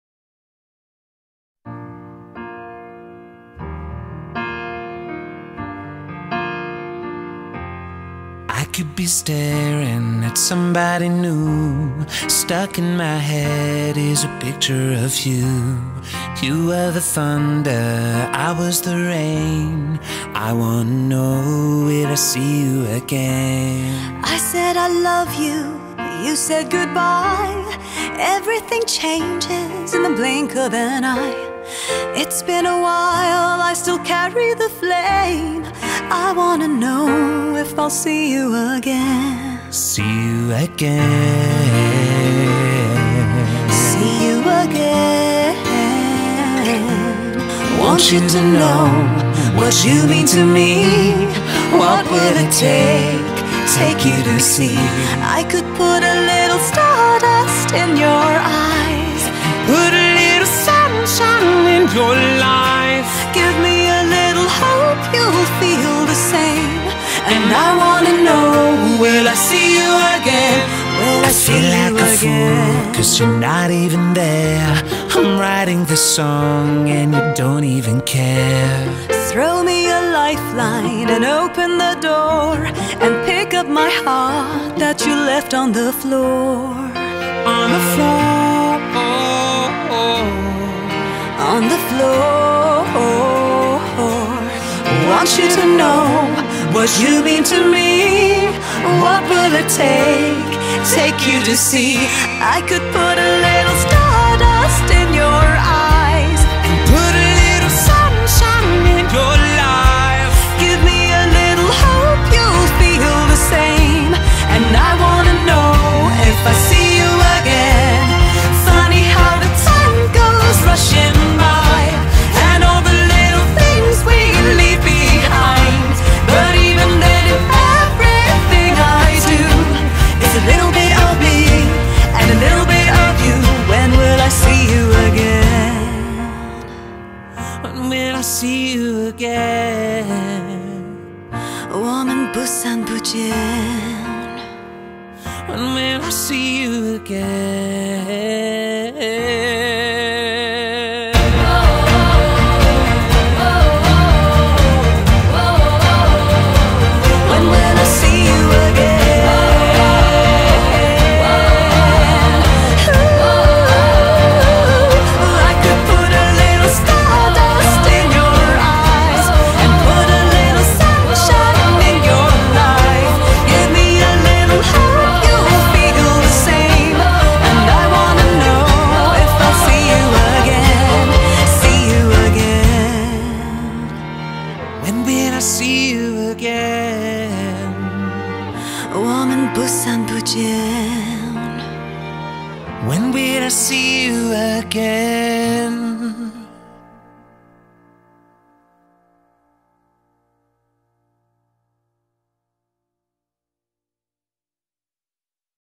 [ipad drawing process] reylo [Star Wars]
Tracked time：27.5h

I suffered a lot drawing this one. spent a lot of time in finding good composition and colors. I may not be very satisfied with the result but still have gained experience during the process. Drawing is both torture and fulfillment.